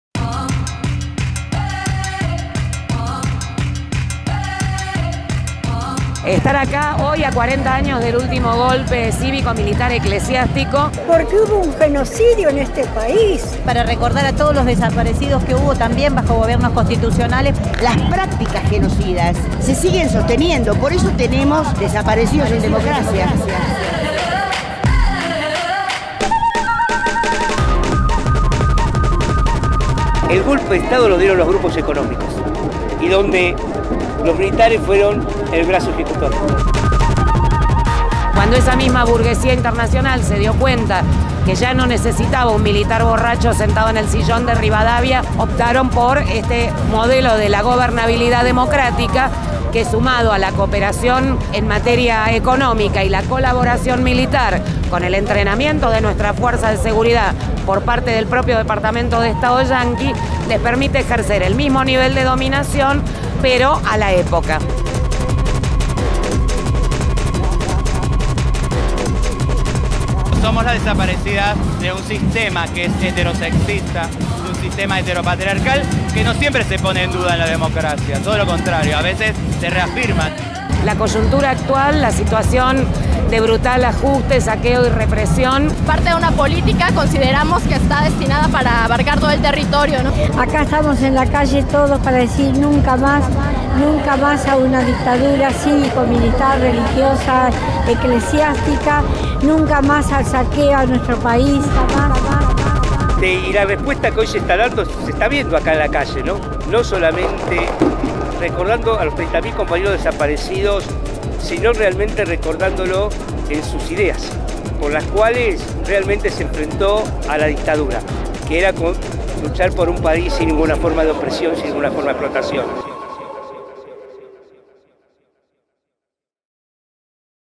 En la calle a 40 años del golpe cïvico, militar, eclesiástico
Para adueñarse de las palabras compraron letras. Quisieron medir las voces y pagaron señales. Miedo para controlar. Acumulan ondas y vocales. Cotizan las noticias. Pretenden disciplinar los cuerpos y redactan protocolos de normalidad. Libertad encorsetada con términos y condiciones. Por eso seguimos silbando, aire que anima el fuego.

Pasaron 40 años del último golpe de estado que detuvo, torturó y desapareció a 30 mil compañeros y compañeras. Años que traen la historia del Juicio a las Juntas, las leyes de Punto Final y Obediencia Debida, los juicios a genocidas por delitos de lesa humanidad, las responsabilidades civiles y complicidades económicas, medios de comunicación concentrados, deuda externa y neoliberalismo.

Otra ronda de jueves en Plaza de Mayo, más nietos y nietas recuperadas, antenas rebeldes y organización en la calle.

Nunca más.

El 24 de marzo - 40 años - Presentes.